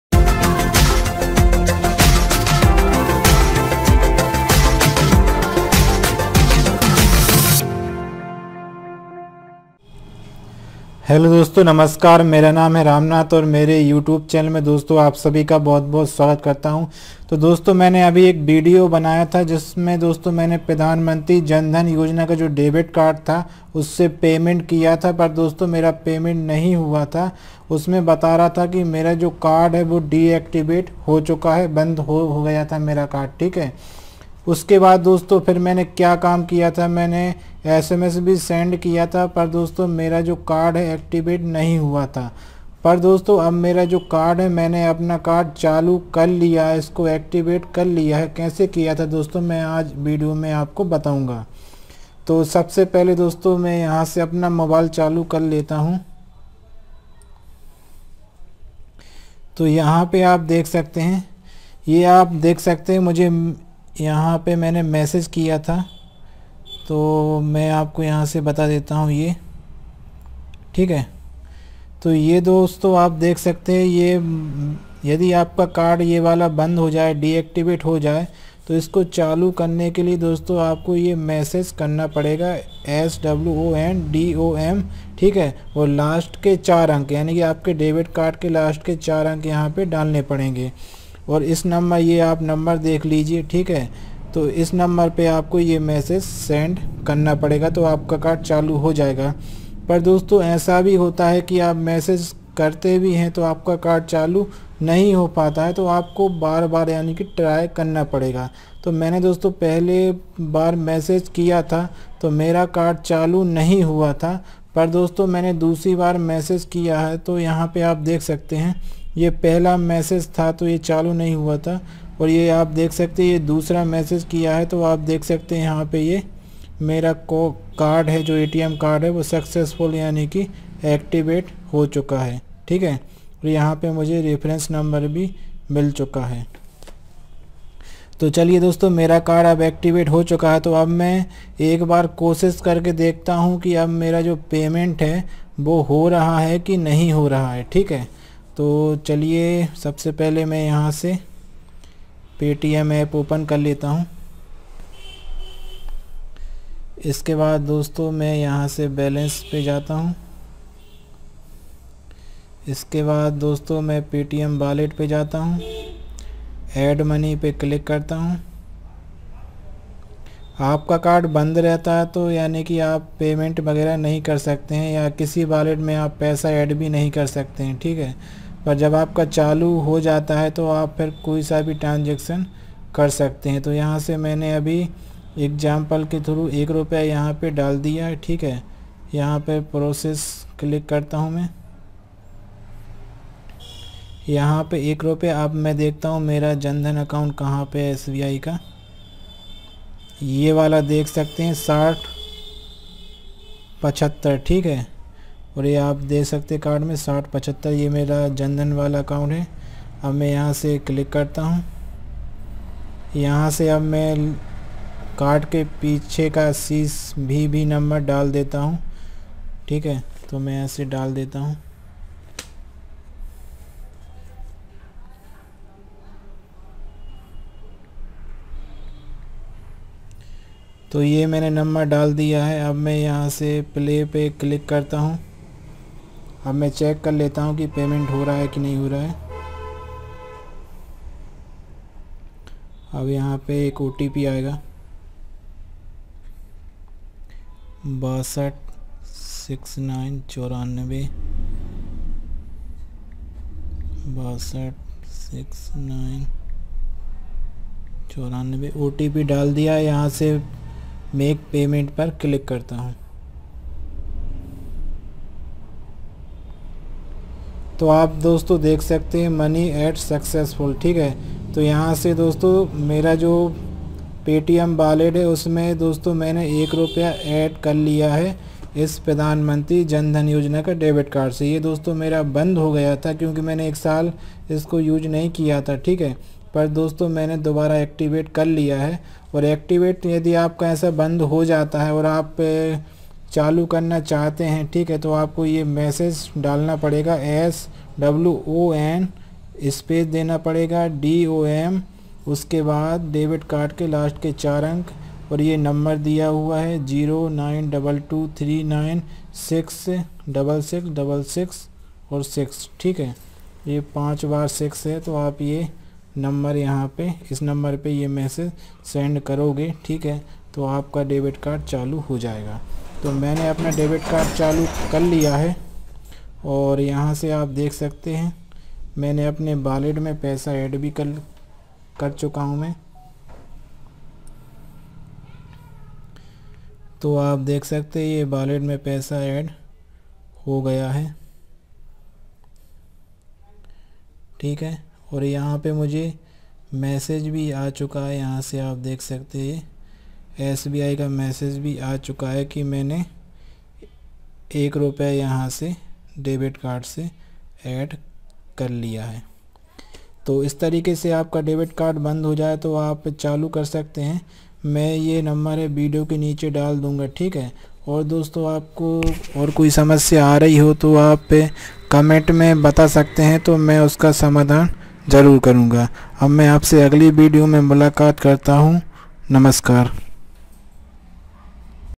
मेरा Debit Card बंद हो गया था मैंने अपना Debit Card चालू कैसे किया ?
debit card चालू करने के लिए आपको ये मेसेजस भेजना पड़ेगा
SWON DOM 1234